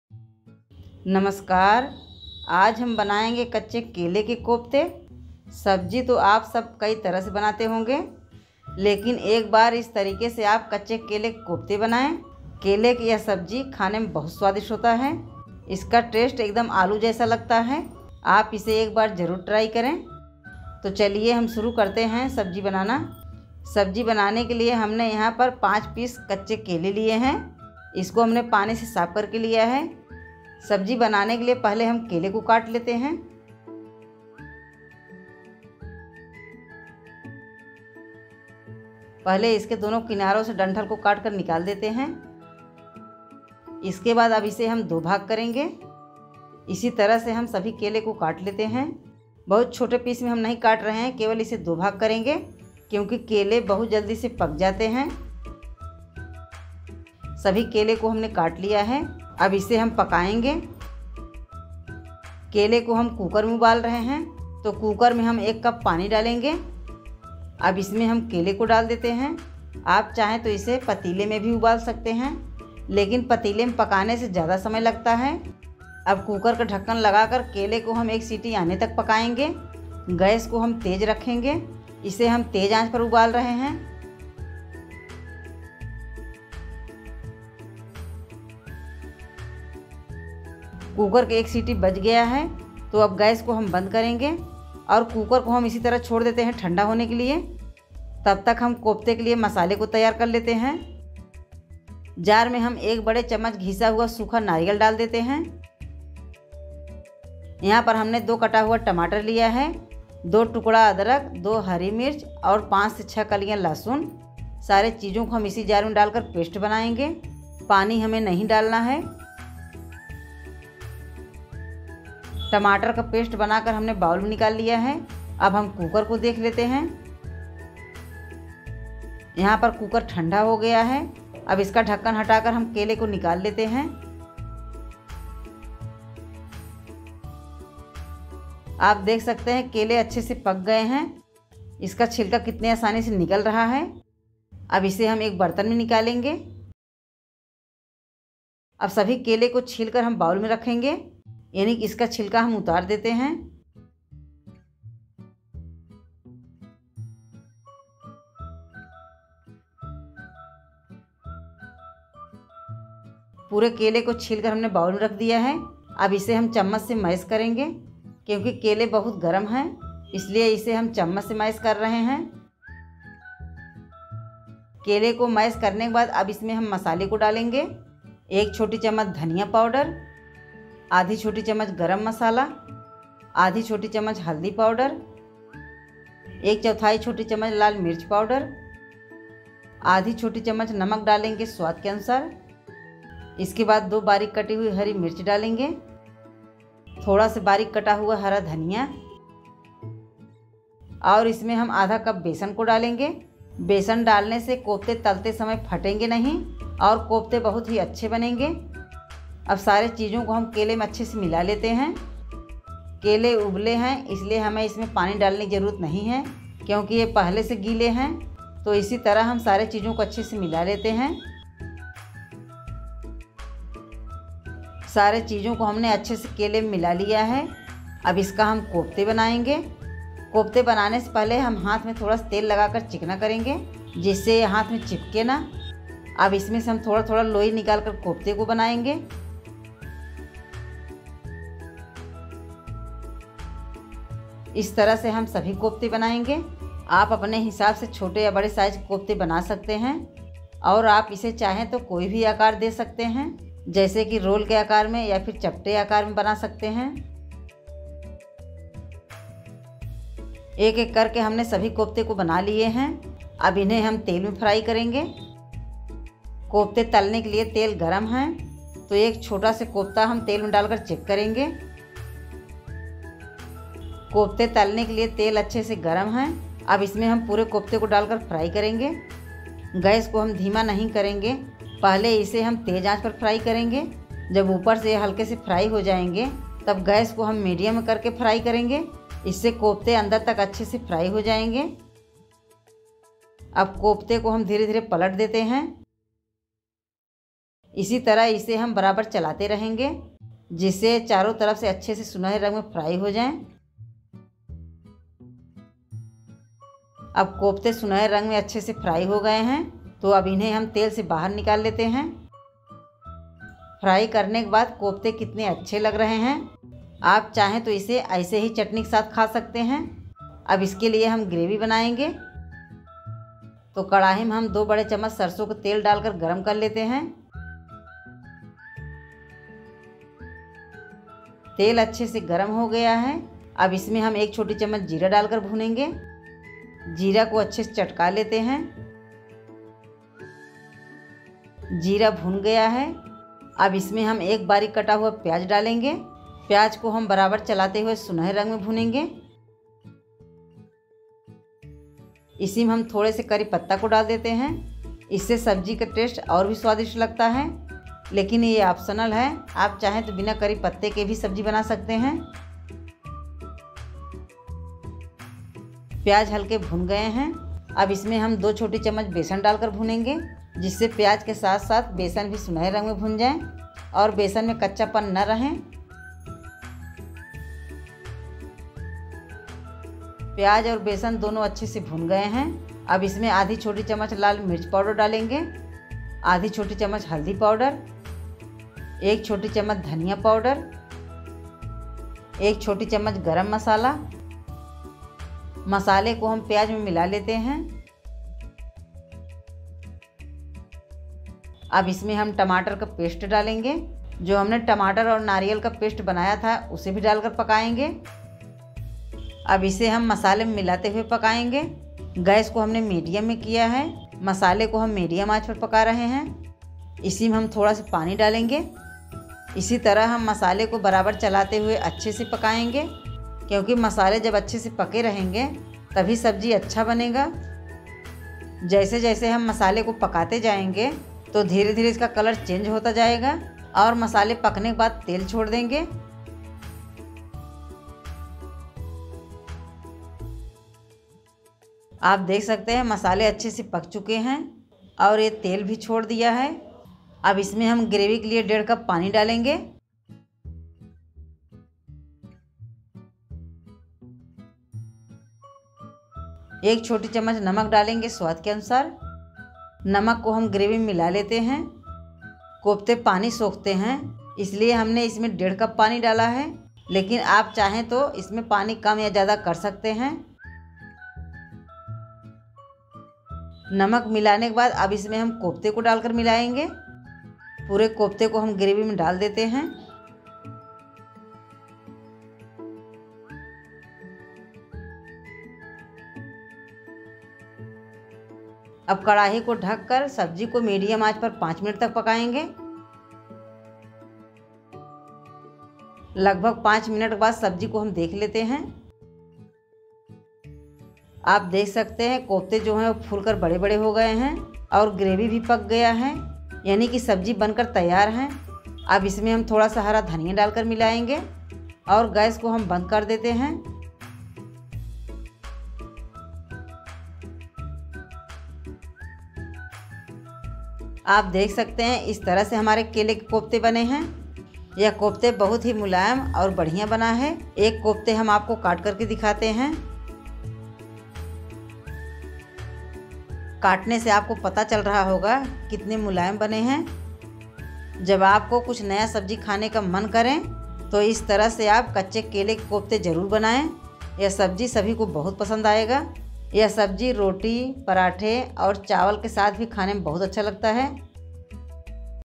कच्चे केले की ऐसी जबरदस्त सब्जी की देखकर ही मुंह में पानी आ जाए | kacche kele ki sabji
kacche kele ki sabji | raw banana vegetable | amma ki thaali | kacche kele ke kofte

Ingredients सामाग्री
Raw Banana कच्चे केले - 5
Grated dry Coconut कद्दूकस सुखा नारियल - 1tbsp
Chopped tomato कटे हुए टमाटर - 2
Ginger अदरक  - 2 (2 inches)
Green chilly हरी मिर्च - 2
Garlic लहसुन - 5 to 6
Red chilly powder लाल मिर्च पाउडर - 1/4 tsp
Salt नमक - 1/2 tsp
Chopped Green chilly कटी हुई हरी मिर्च- 2
Some coriander leaves कुछ धनिया पत्ती
Gram flour बेसन- 1/2 cup - 100gm
Cooking oil तलने के लिए तेल

For sabji gravy सब्जी ग्रेवी
Mustard oil सरसो तेल - 2 tbsp
Cumin seeds जीरा - 1 tsp
Chopped onion प्याज - 1
Some curry leaves कुछ करी पत्ता
Gram flour बेसन- 2 tsp
Red chilly powder लाल मिर्च पाउडर- 1/2 tsp
Garam masala powder गरम मसाला पाउडर - 1 tsp
Salt नमक - 1 tsp to taste

🪙Check my another recipe